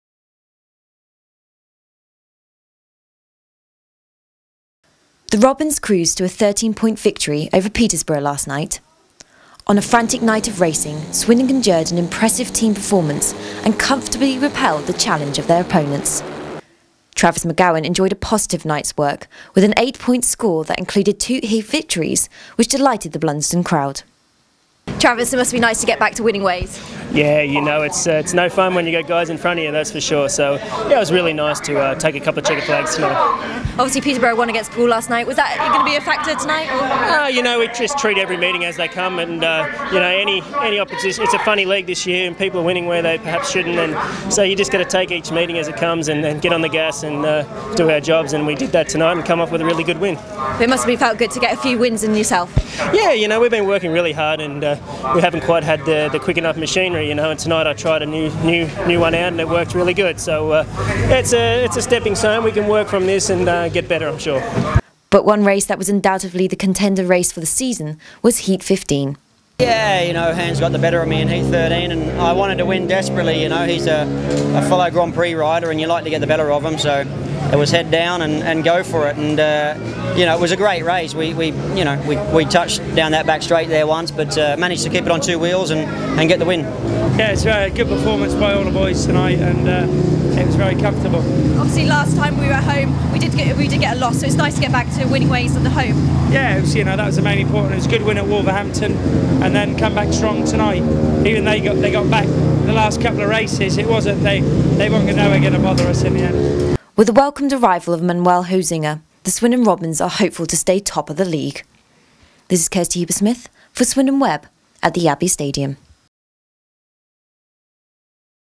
Swindon Robins 53 Peterborough 40
Swindon stretch their lead at the top of the Sky Sports Elite League...